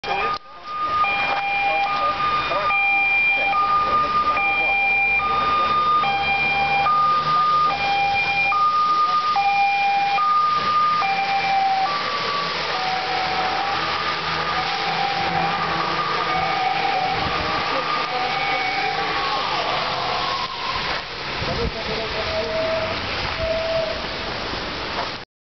301[G]49- przejazd alarmowy- ul. Słowackiego
Wóz z jednostki JRG 1 Gdańsk. Wóz przyjechał pomagać przy usuwaniu skutków powodzi. Podziękowania za fontannę :)